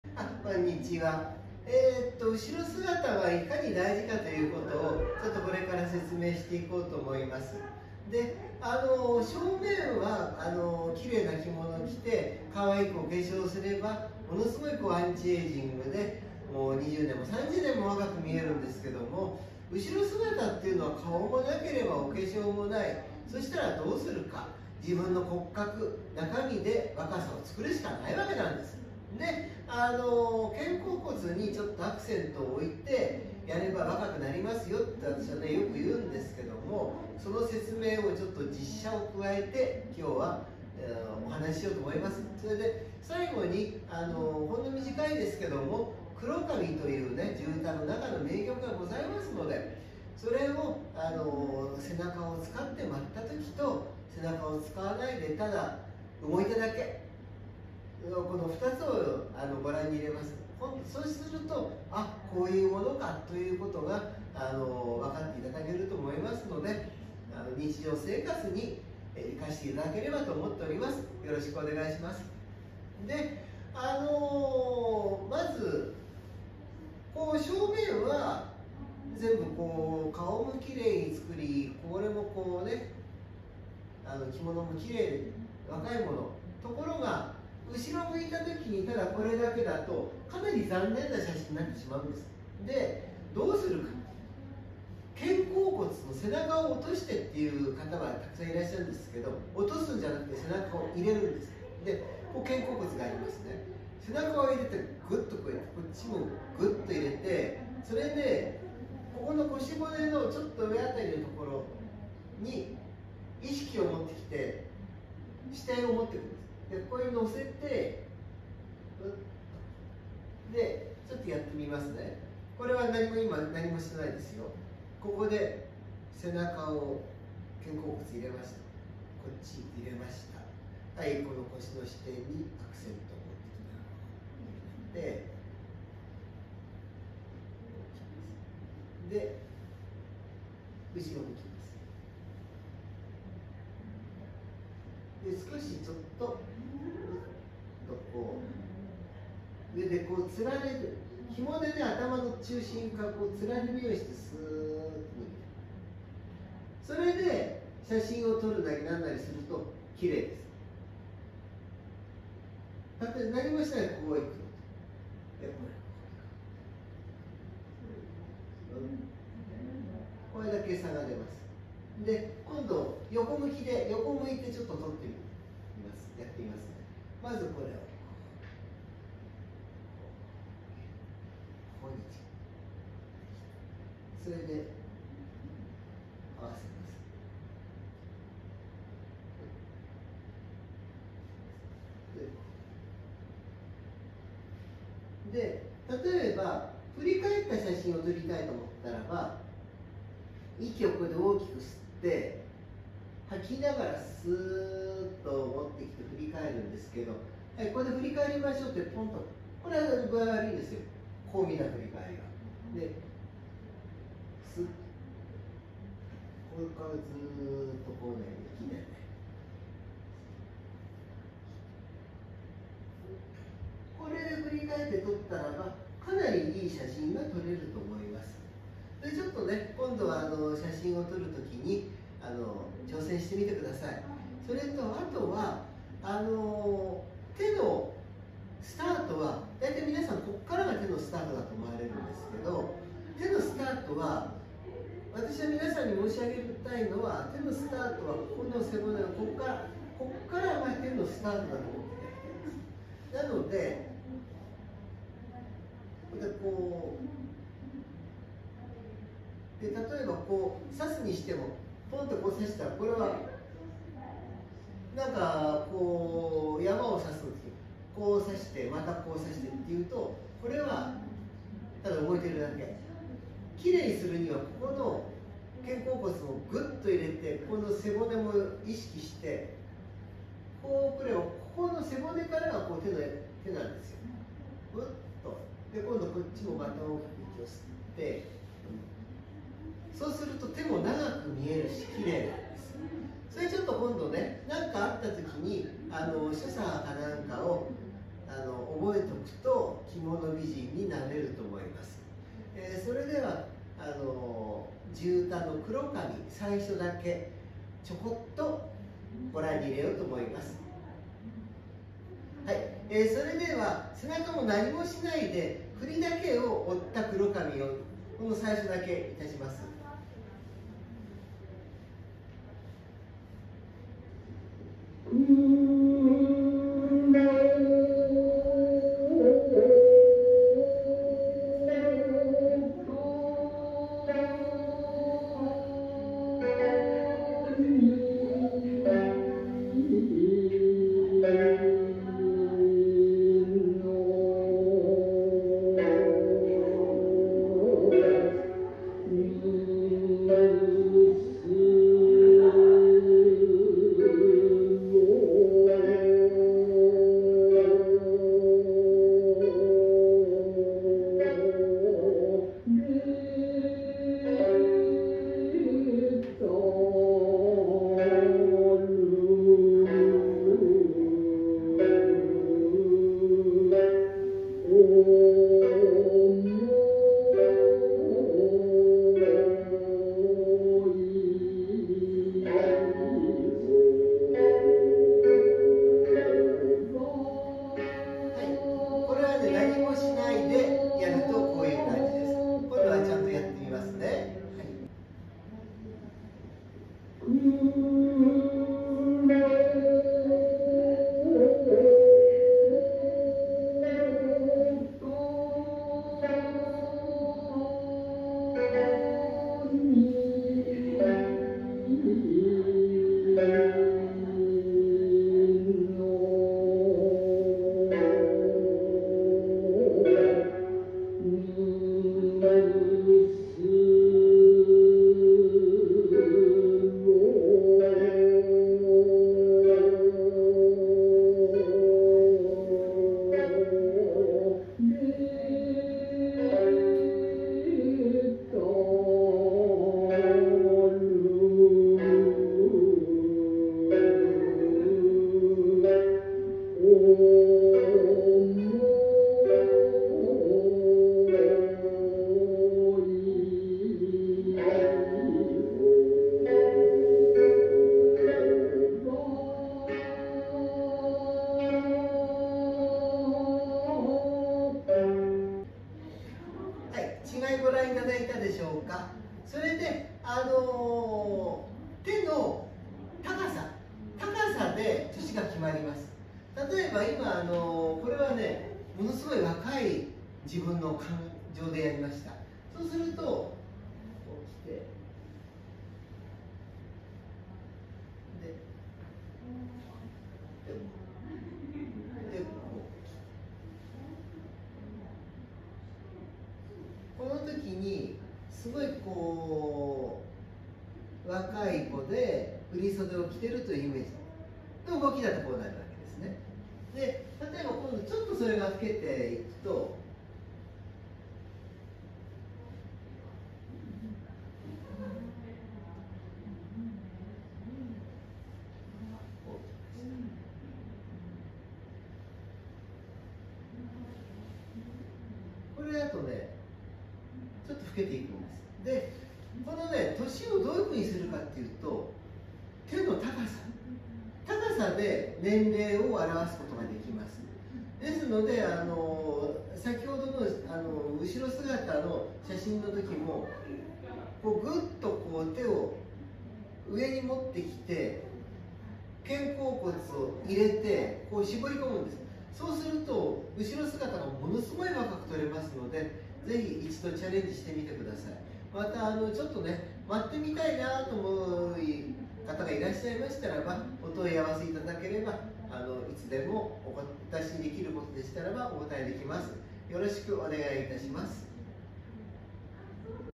美しく魅せるために ゴーイング舞ウェイ
日常生活に役立つ所作を舞を通して紹介していきます。